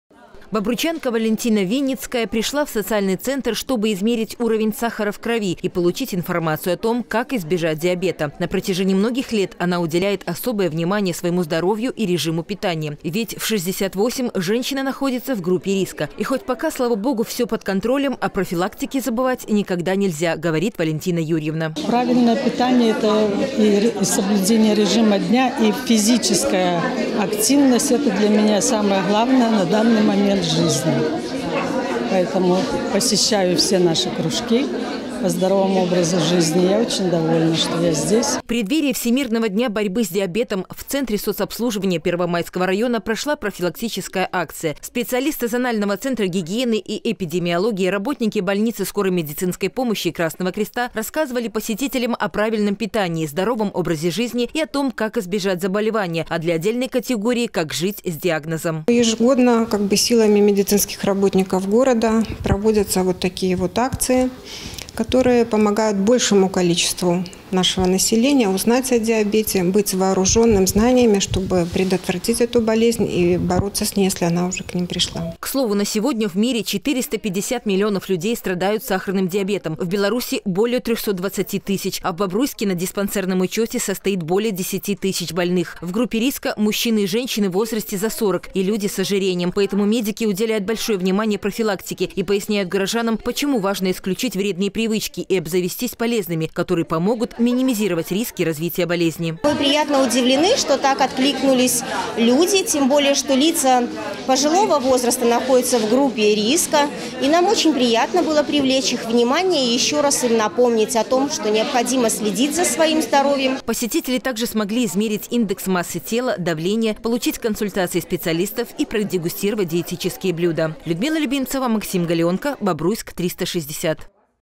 Самоконтроль – путь к здоровью!
В Центре соцобслуживания населения Первомайского района прошла акция, посвященная всемирному дню борьбы с диабетом, который отмечается сегодня.